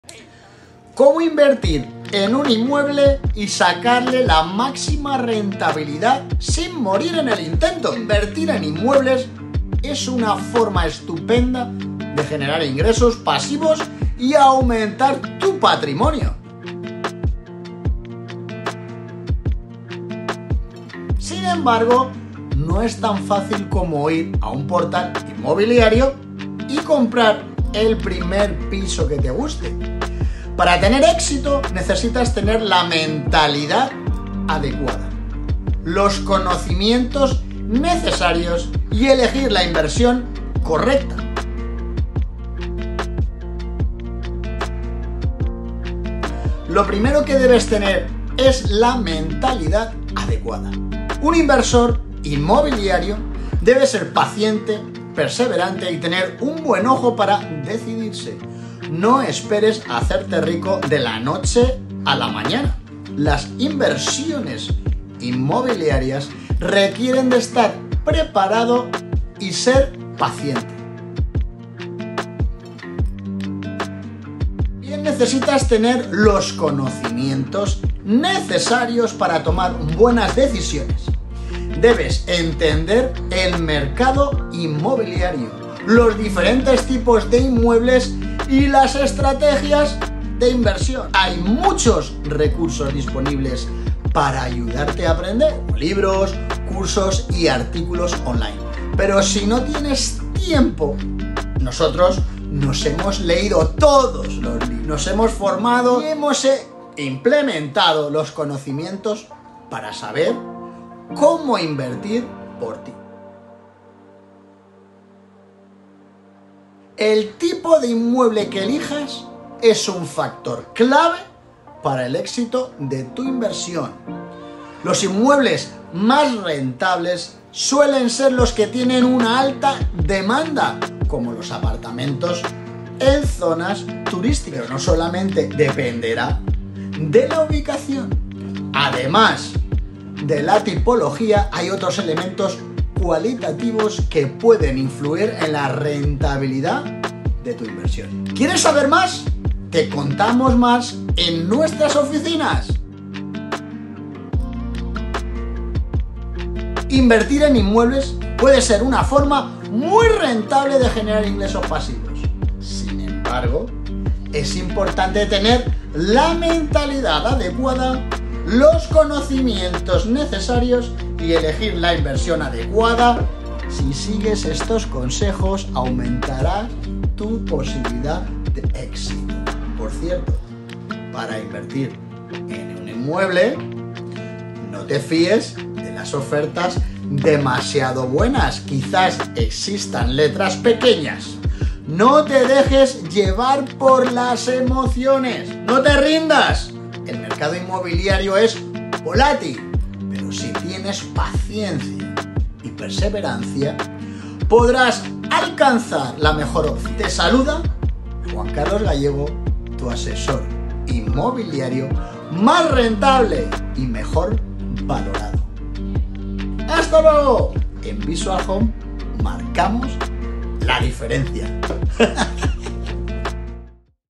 Las claves para invertir y rentabilizar tu vivienda
Convierte tu inversión en la forma de mejorar tu calidad de vida con las claves que te voy a contar en estos tips 😉
Haz que tu inversión sea rentable y disfrutes de ella 🚀🏘️